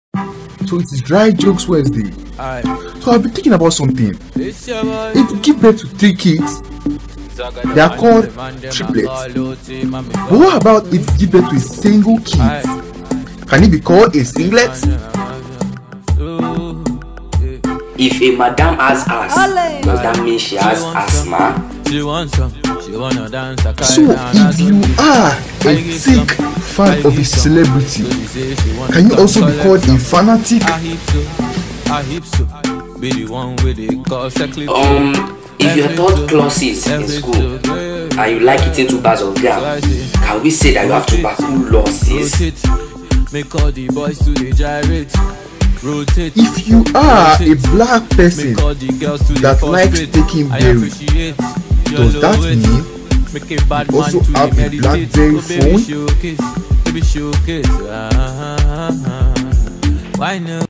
Comedy (Dry jokes😂😂😁😀😄)
A comedy combination of dry jokes by acecomedian and naijaclown